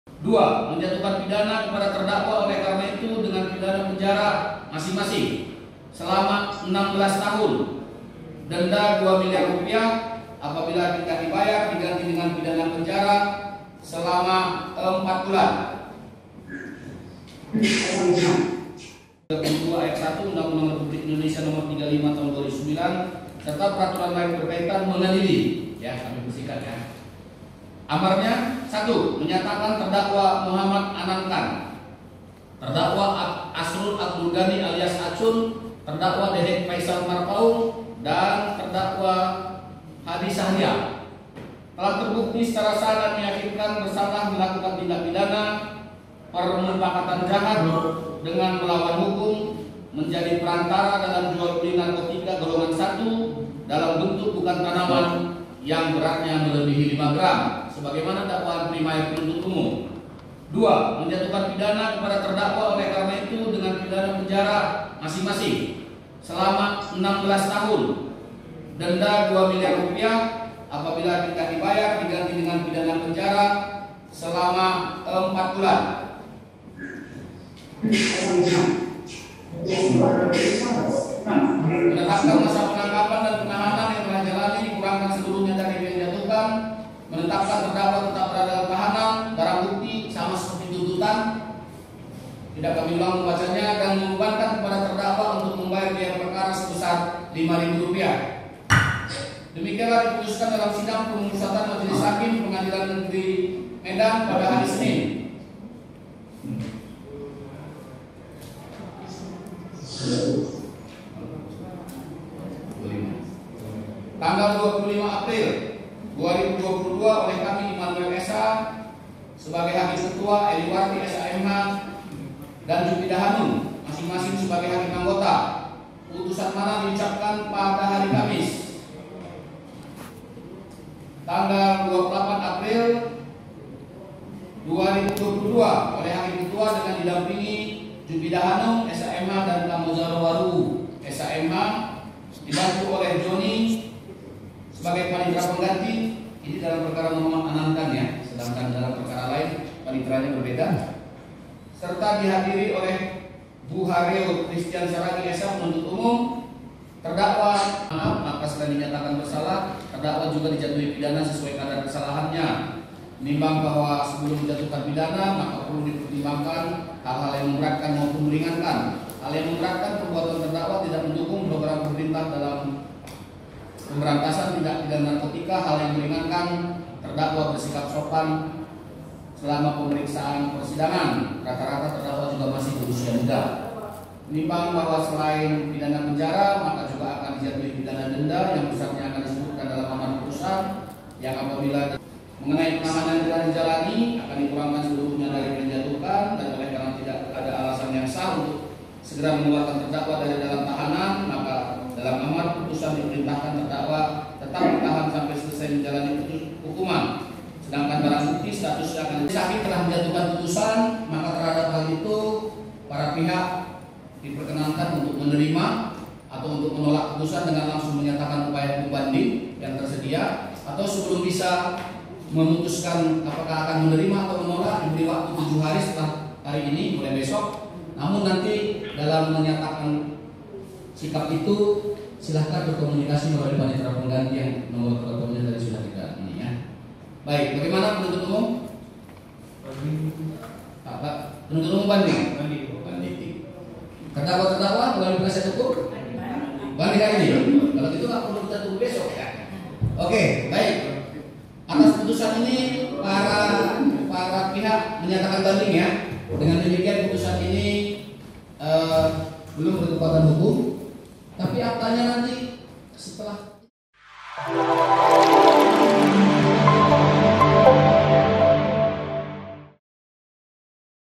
DIVONIS 16 Tahun Penjara, Kurir Narkoba asal Tanjungbalai Menangis
TRIBUN-MEDAN.com, MEDAN - Divonis 16 tahun penjara, tiga kurir sabu asal Tanjungbalai menangis sesegukan di Pengadilan Negeri Medan, Kamis (28/4/2022).

Adapun keempat terdakwa yakni Dedek Faisal Marpaung, Hadi Syahrial, Asrul Abdul Gani alias Acun dan satu warga Deliserdang Muhammad Anand Khan.

Majelis Hakim yang diketuai Immanuel Tarigan menilai, keempat terdakwa terbukti bersalah menjadi kurir narkotika jenis sabu seberat 7 kg.

"Menjatuhkan pidana kepada para terdakwa dengan pidana penjara masing-masing selama 16 tahun, denda Rp 2 miliar, apabila tidak dibayar diganti 4 bulan penjara," kata hakim.